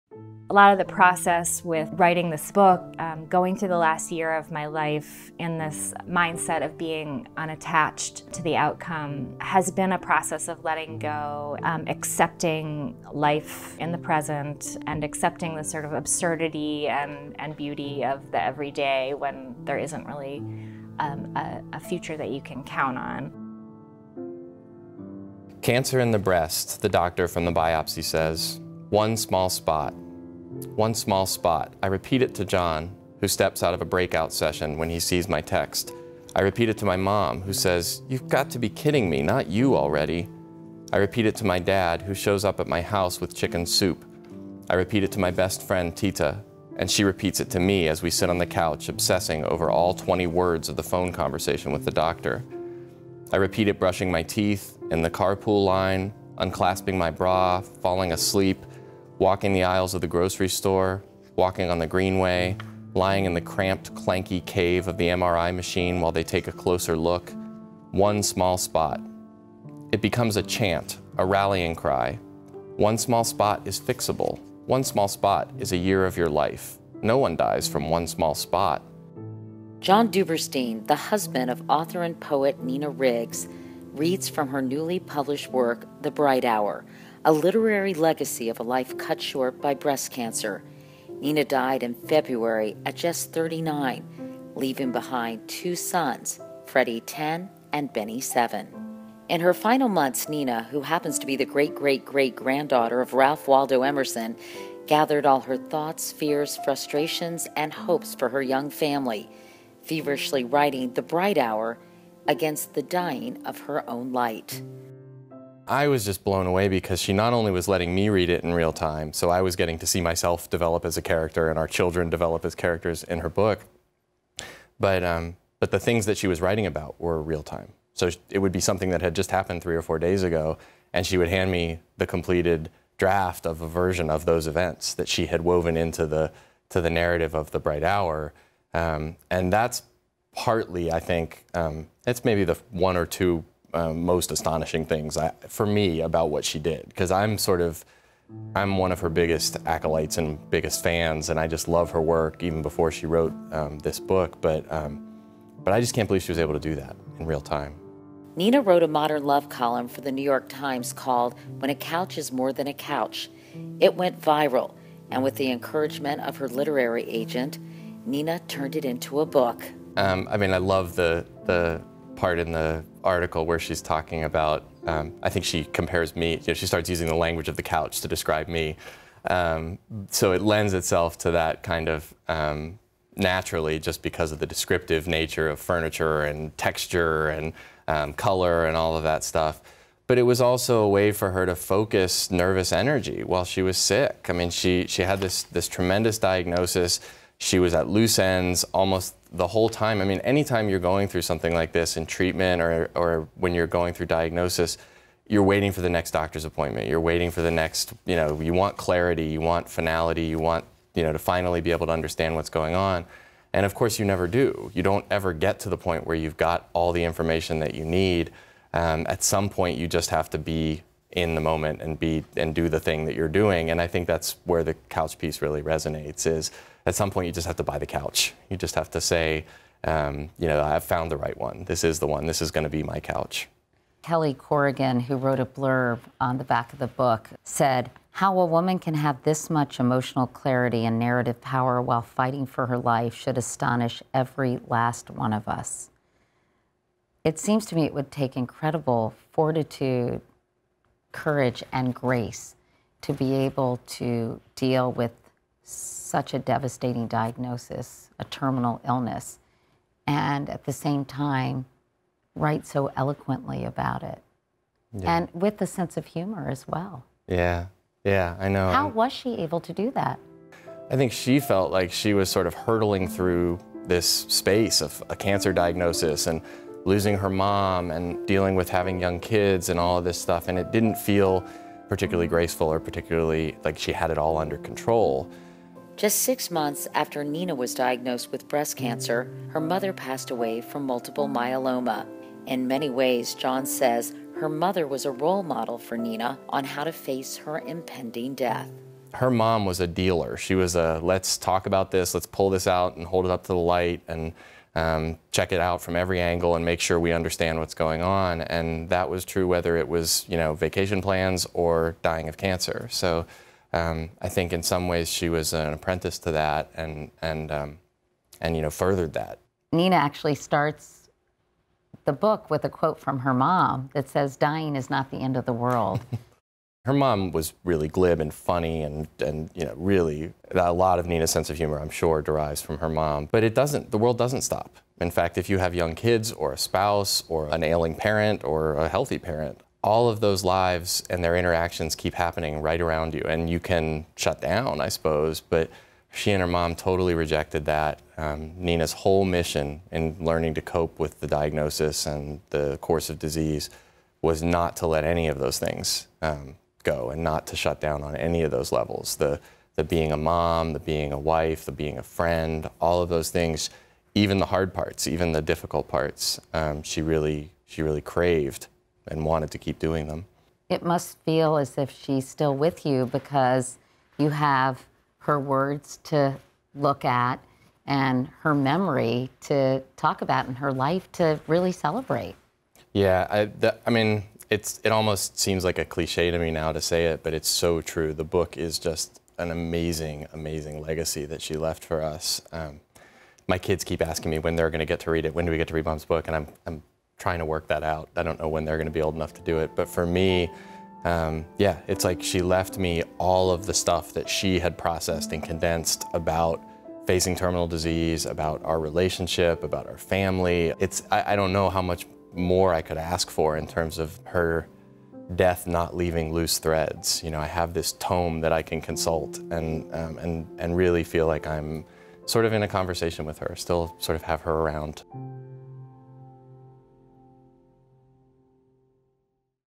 Late author inspires others to accept life in the present in new memoir 'The Bright Hour'
Following a breast cancer diagnosis, Nina Riggs spent her final months gathering all her thoughts, fears, frustrations and hopes for her young family into “The Bright Hour: A Memoir on Living and Dying.” She passed away in February, but her memoir will live on forever.